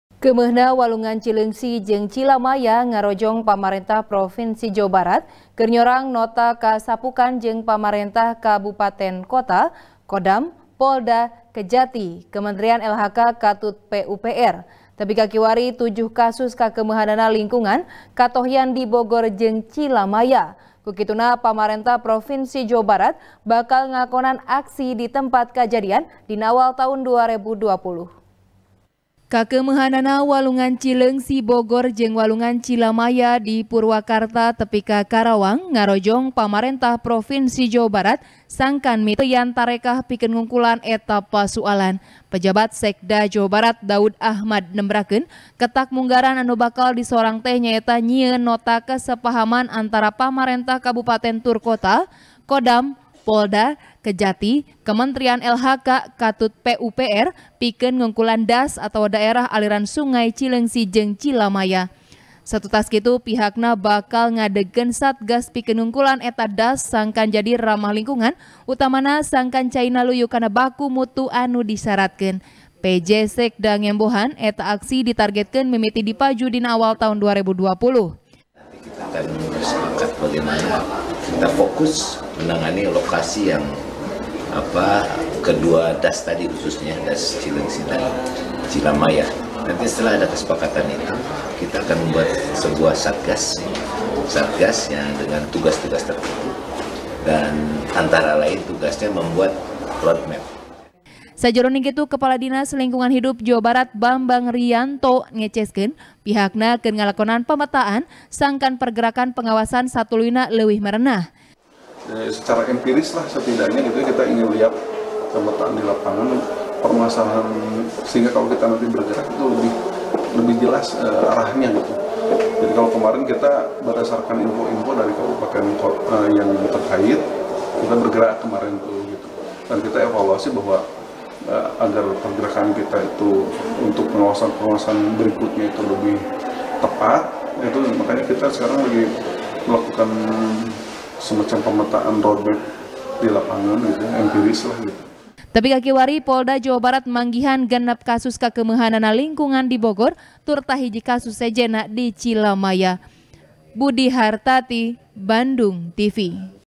Walungan Cileungsi Kakeumeuhan, Aksi Disorang Pemprov Awal Taun 2020 | TP | BANDUNGTV
TP | ELBY NOPITRI | Keumeuhna Walungan Cileungsi Jeung Cilamaya Ngarojong Pamarentah Provinsi Jawa Barat Keur Nyorang Nota Kasapukan Jeung Pamarentah Kabupaton Kota, Kodam, Polda, Kejati, Kementerian L-H-K Katut P-U-P-R. Tepi Ka Kiwari, Tujuh Kasus Kakeumeuhanana Lingkungan, Katohyan Di Bogor Jeung Cilamaya. Ku Kituna, Pemprov Jabar Bakal Ngalakonan Aksi Di Tempat Kajadian, Dina Awal Taun Dua Rebu Dua Puluh.